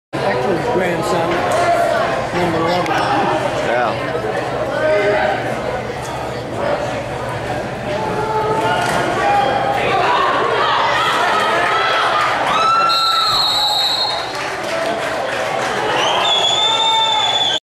Justin Caroll's td
Justin takes one in on the first play from scrimage.